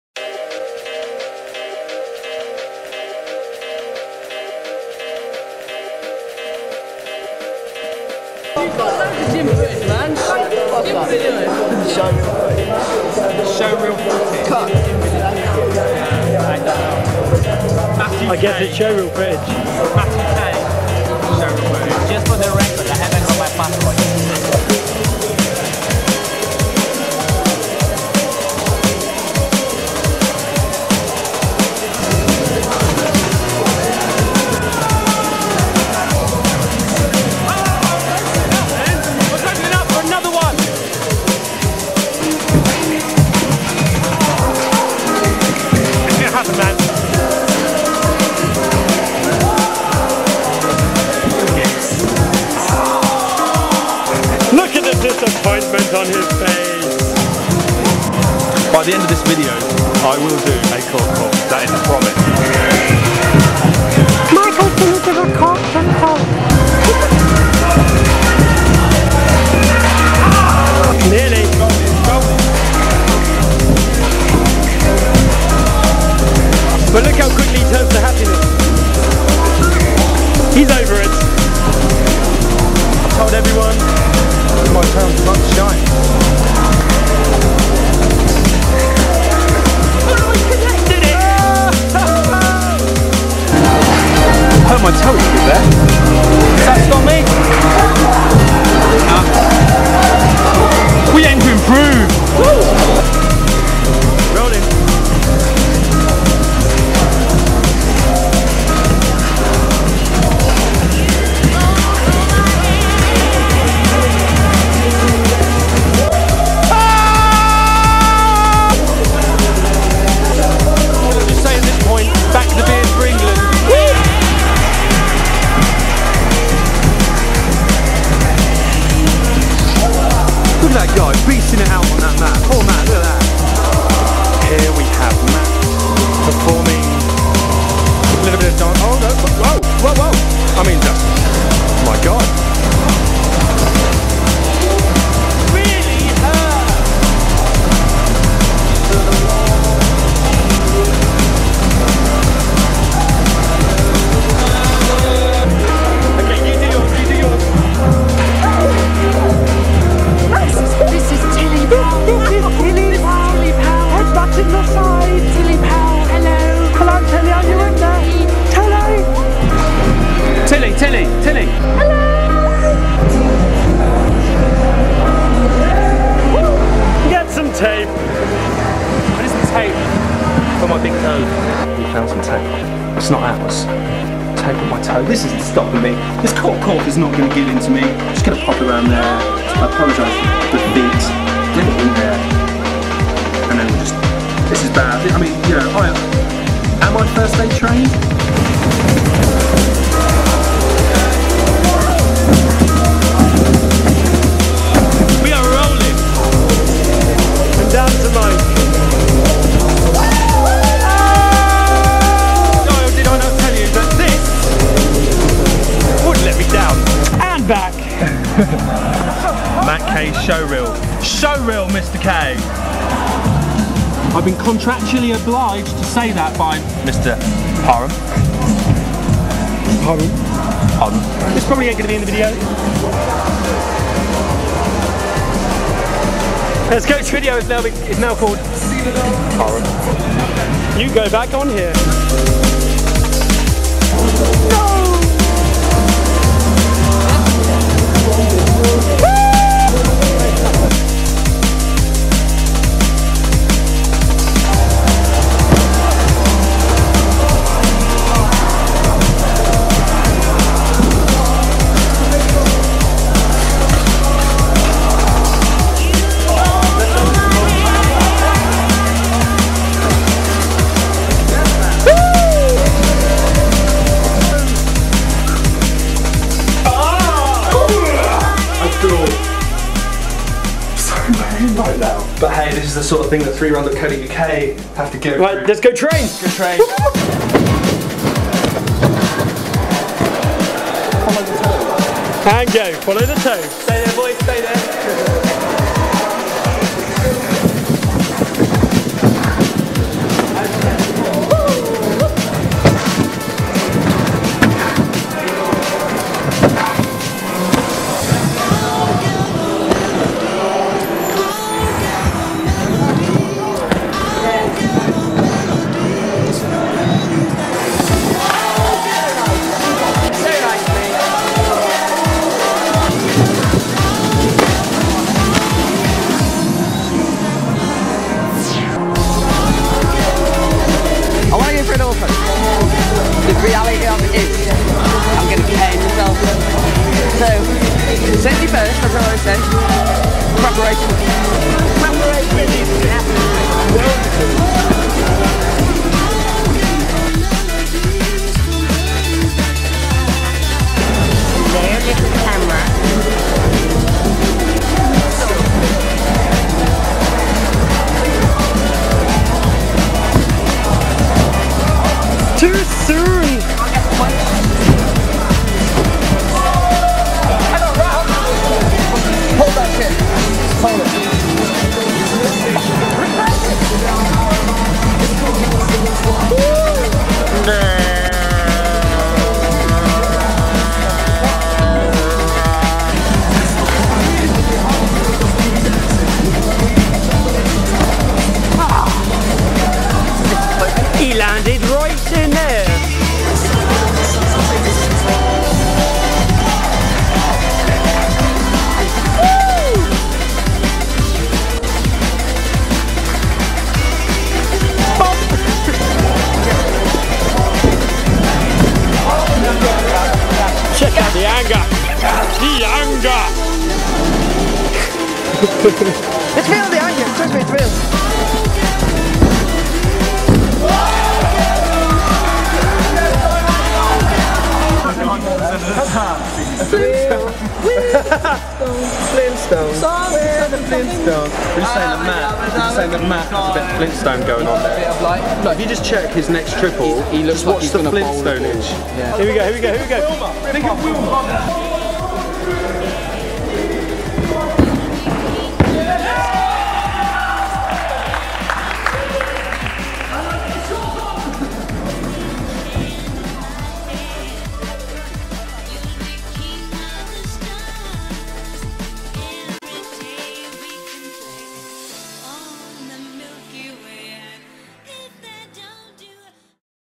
Get showreel footage for Mat
Got a new Lens for my camera set out to get some showreel footage but ended up having so much fun training :P so decided to make a video in a day including editing Finished at 7am this morning! but I did it!!  A amazing gym session captured on film like never before!